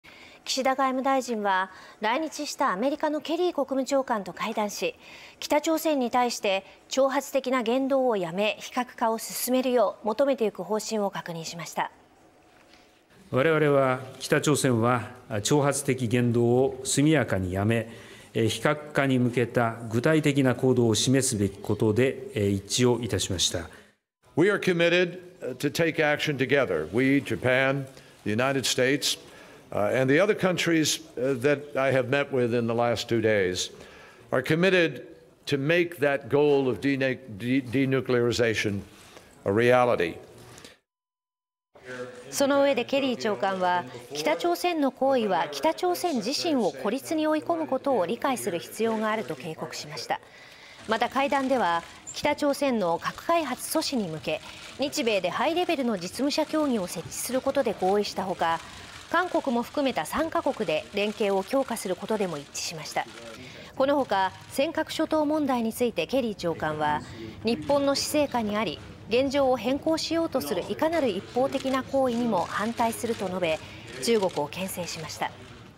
対"北"連携確認 岸田外務大臣と米国務長官会談(13/04/15)
岸田外務大臣は来日したアメリカのケリー国務長官と会談し、北朝鮮に対して、挑発的な言動をやめて非核化を進めるよう求めていく方針を確認しました。